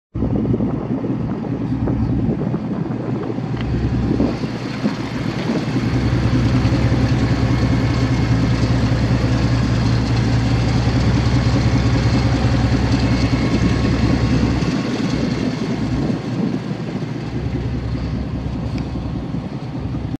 1998 Dodge Ram 1500 Club Cab pickup truck for sale at auction | bidding closes February 27, 2019
1998 Dodge Ram 1500 Club Cab pickup truck
176,034 miles on odometer
5.2L V8 OHV 16V gas engine
AM/FM
LT265/75R16 tires
End gate lift
Unknown operating condition
Dead battery
Missouri title
3B7HF12Y5WM216295
It's located in Bethel, MO, and we're selling it to the highest bidder regardless of price.